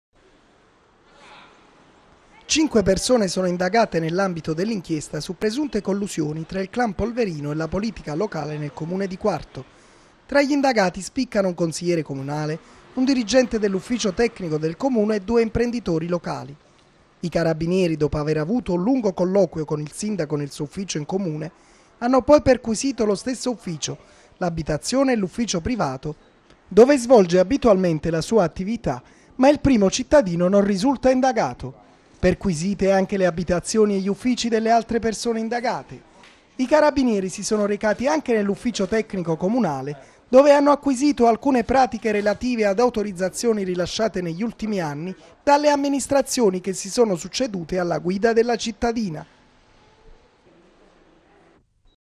Quarto (NA) - Camorra, perquisizioni in comune e a casa del sindaco (09.07.12)
- Quarto (NA) - Camorra, perquisizioni in comune e a casa del sindaco. Cinque persone sono indagate nell'ambito dell'inchiesta su presunte collusioni tra il clan Polverino e la politica locale nel comune di Quarto. Tra gli indagati spiccano un consigliere comunale, un dirigente dell'ufficio tecnico del Comune e due imprenditori locali. I carabinieri dopo aver avuto un lungo colloquio con il sindaco nel suo ufficio in Comune hanno poi perquisito lo stesso ufficio, l'abitazione e l'ufficio privato dove svolge abitualmente la sua attività ma il primo cittadino non risulta indagato. Perquisite anche le abitazioni e gli uffici delle altre persone indagate. I carabinieri si sono recati anche nell'ufficio tecnico comunale dove hanno acquisito alcune pratiche relative ad autorizzazioni rilasciate negli ultimi anni dalle amministrazioni che si sono succedute alla guida della cittadina. (09.07.12)